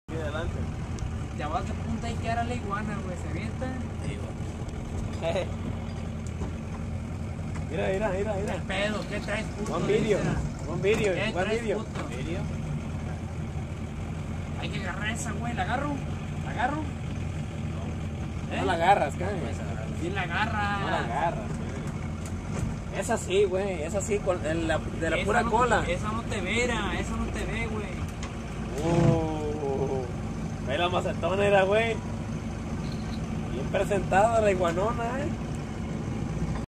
Iguanas verdes gigantes
This is an everyday view of the wild iguanas in Puerto Vallarta. They are cold blodded so they need the suns energy to digest their food. Mangle or Mangrove is very interesting because it separates the salt from the water in saltwater. It also serves very well as protection agains erosion.
Iguanas verdes gigantes en los canales de Nuevo Vallarta Nayarit.